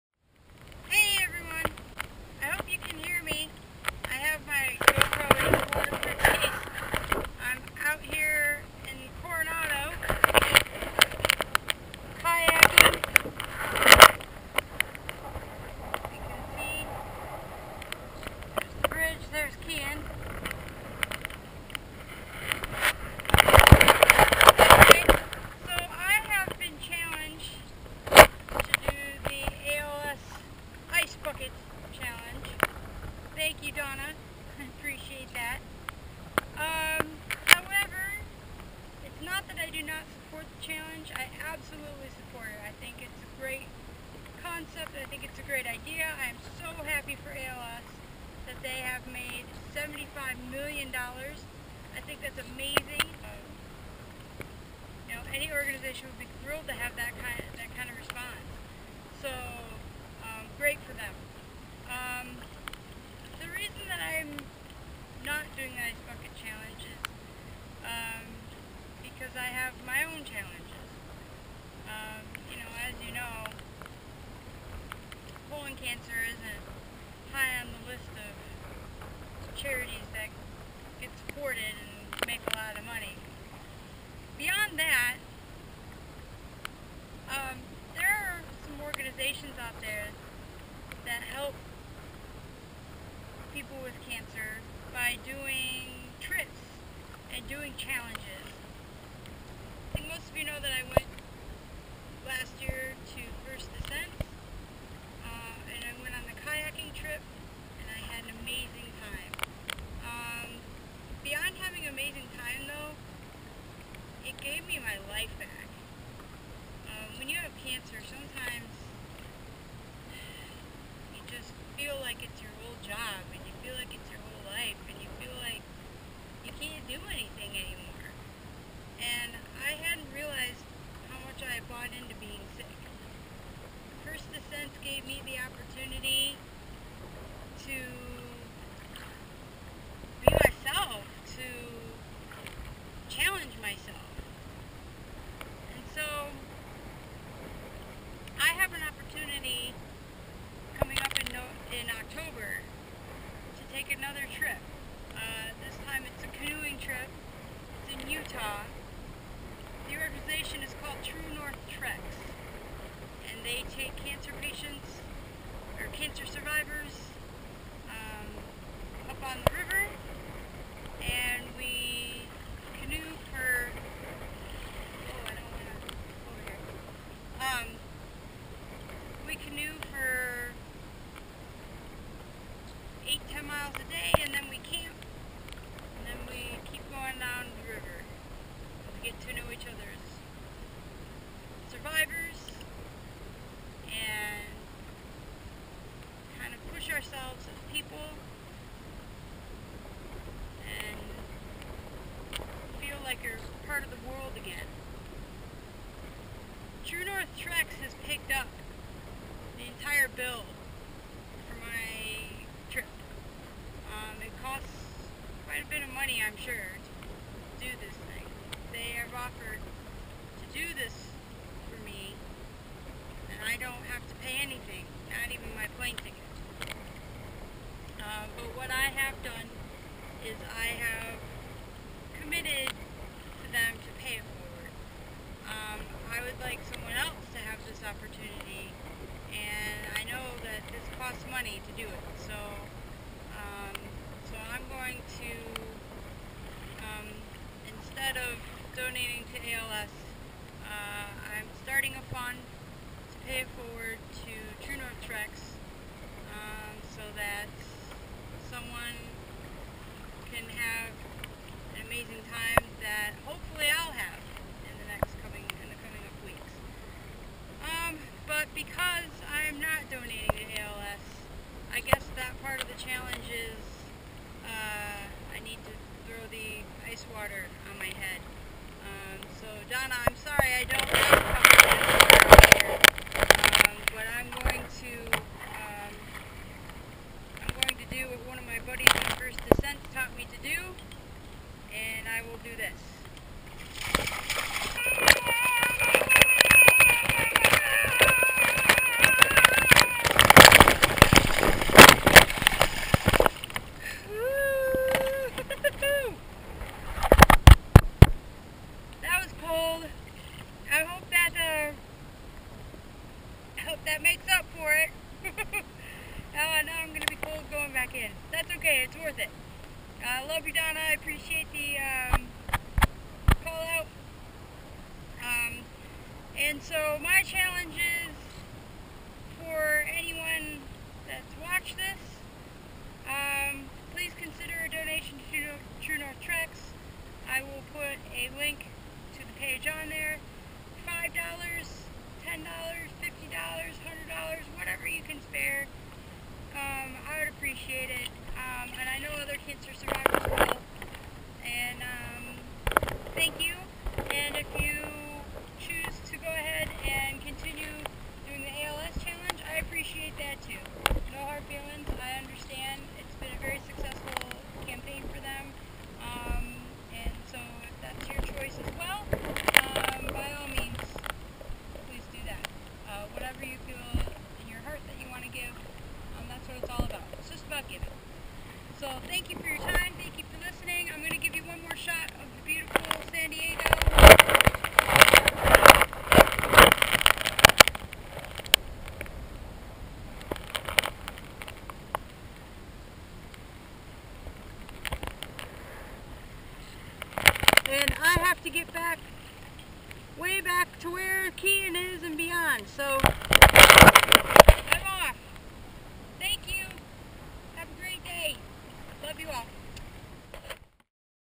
TNTVideo
This video is about why I'm declining doing the ice bucket challenge.

I'm doing the "Show me your belly"challenge instead,